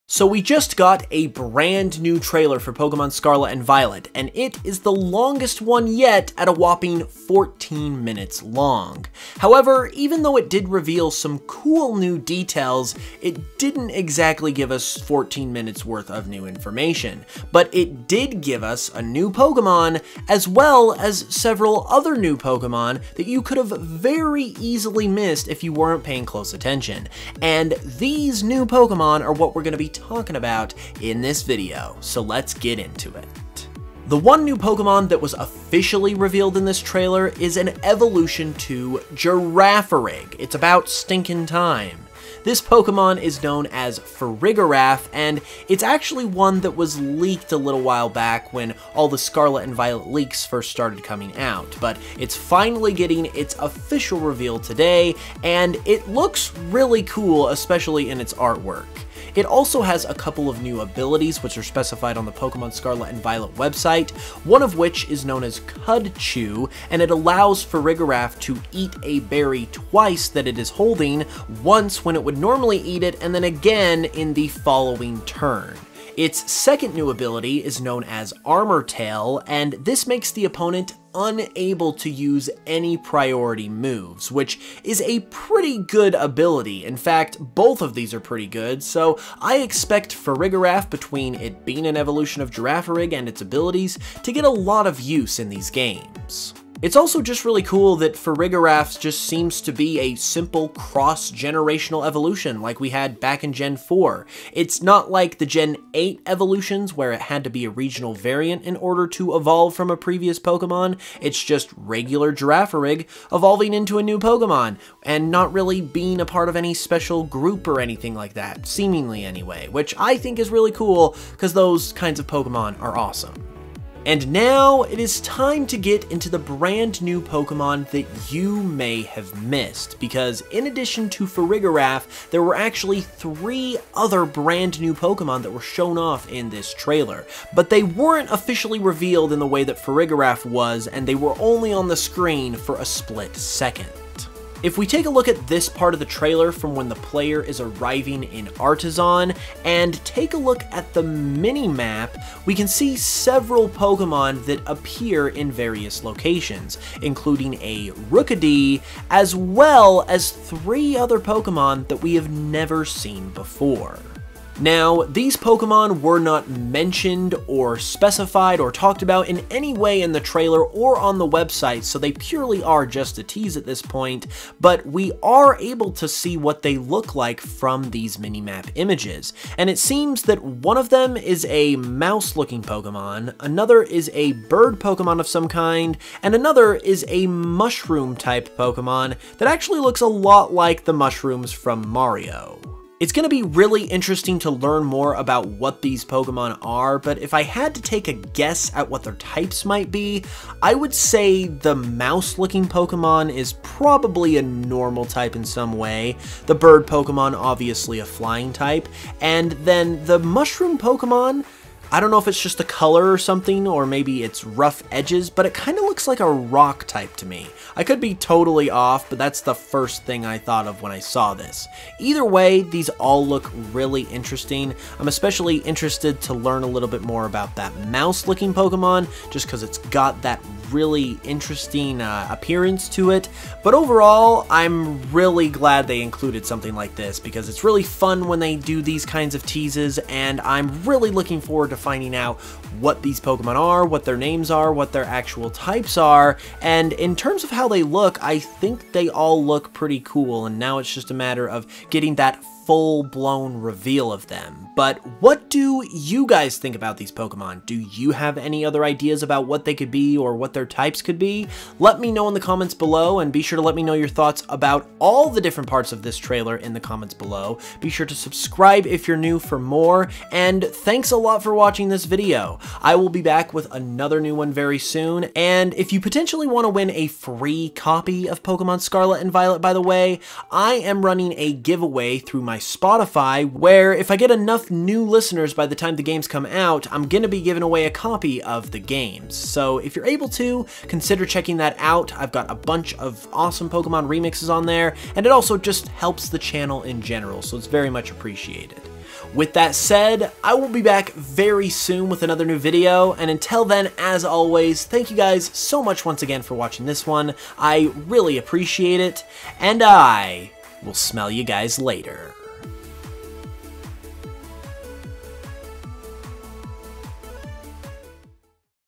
SECRET NEW Pokémon Revealed in the New Pokémon Scarlet and Violet Trailer! 👀
A new trailer was just released for Pokemon Scarlet and Violet, which not only revealed a brand new Girafarig evolution named Farigiraf, but also a few secret new Pokemon that snuck their way into the trailer as well, which you could have easily missed! In this video we'll be covering and discussing all these new Pokemon, so I hope you enjoy!